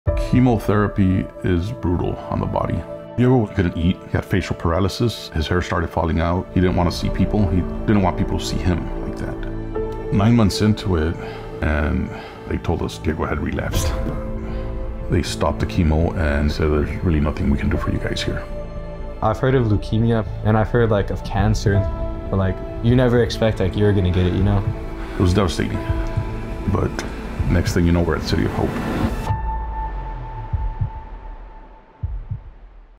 Teen Leukemia Patient Thrives with Help from Supportive Care
When City of Hope Children’s Cancer Center patient Diego Serna was diagnosed with an aggressive form of leukemia, his world turned upside down. The physical and emotional toll of treatment was often overwhelming, leading the teen to isolate himself and leaving his family feeling helpless.

But thanks to City of Hope Children’s Cancer Center and The Sheri and Les Biller Family Foundation, Diego received the counseling and emotional support he needed to navigate his cancer journey and improve his quality of life. Diego and his dad, Isaias, discuss the challenges he faced and how supportive care became a lifeline when he needed it the most.

This film was produced for City of Hope by @BBCStoryWorks Commercial Productions.